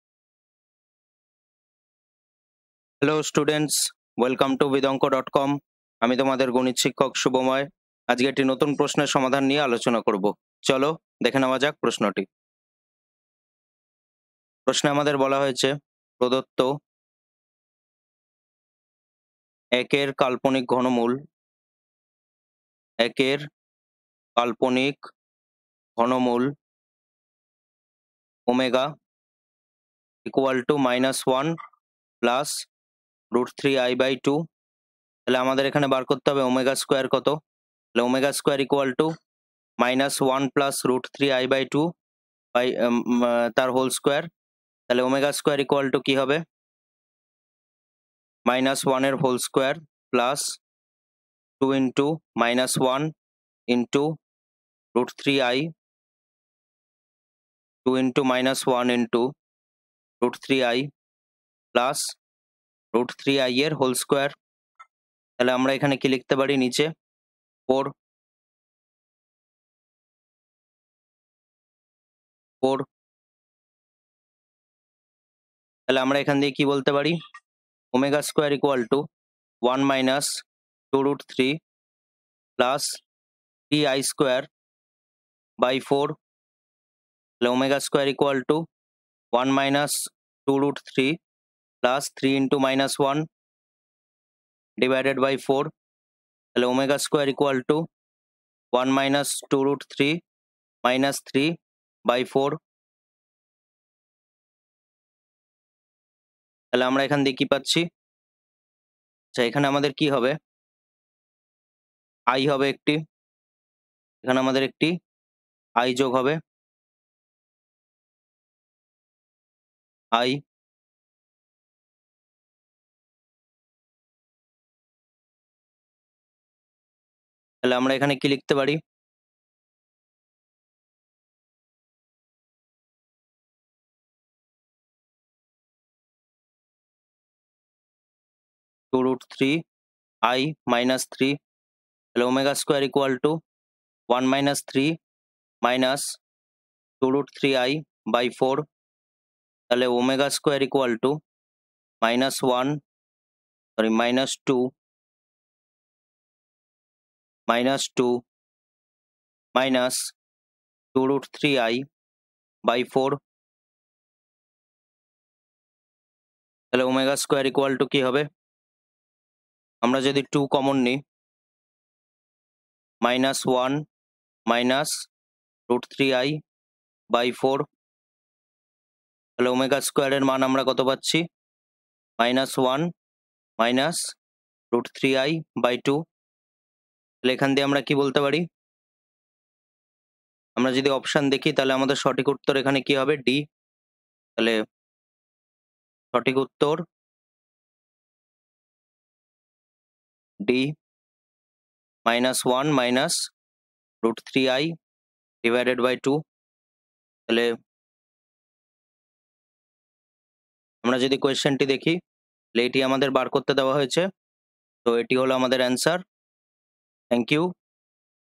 WB Board 11th Class 11 Math Book Solution in Bengali - S N Dey Exercise Question: 15.1.10
Get free Maths book video and PDF solutions in Bengali for S N Dey. Free answer and explanation through video for question 15.1.10, of Chapter Complex Number from S N Dey followed for Class 11, West Bengal Council of Higher Secondary Education (WBCHSE).